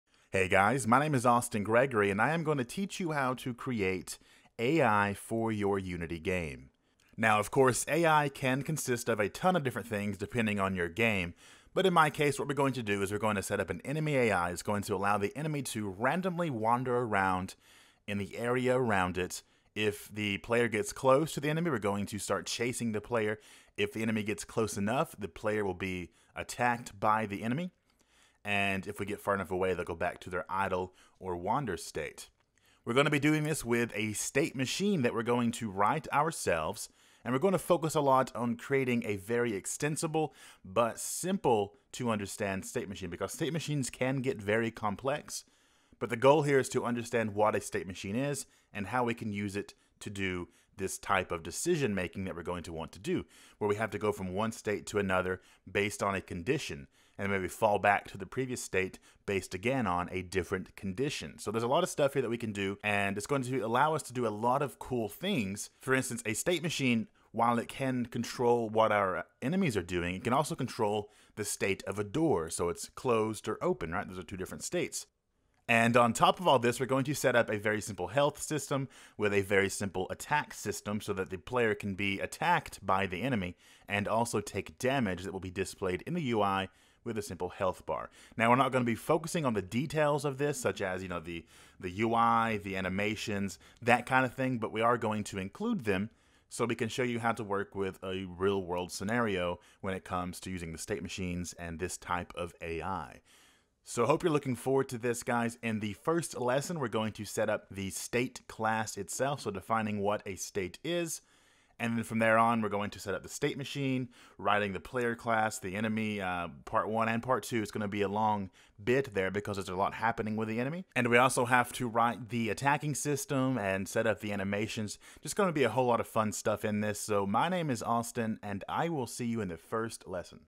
How to Use AI and State Machines in Unity Games
Develop your first state machine from scratch. Learn to create smart enemies for your games!

Create an enemy AI that wanders randomly until players get close, chases players within a certain range, attacks players within close range, and returns to its wandering state when players get far enough away... completely from scratch!